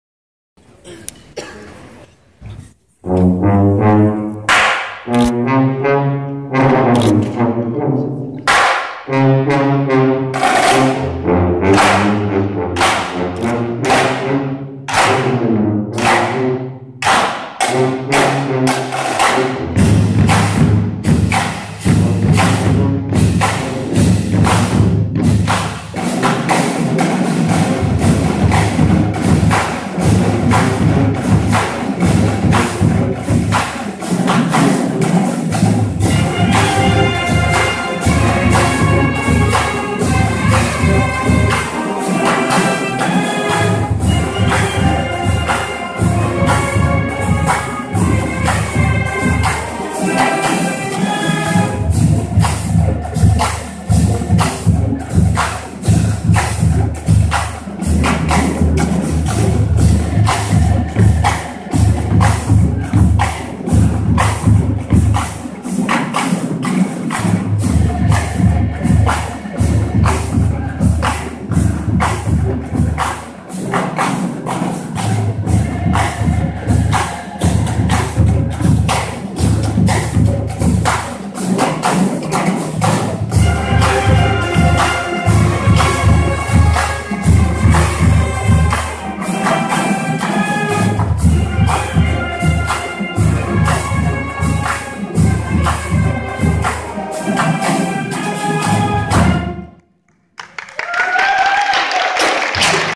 Lord Mayor Enters to WBHS Vanguard
Duncan Sandys, former Lord Mayor of Westminster, enters the West Boca High theater.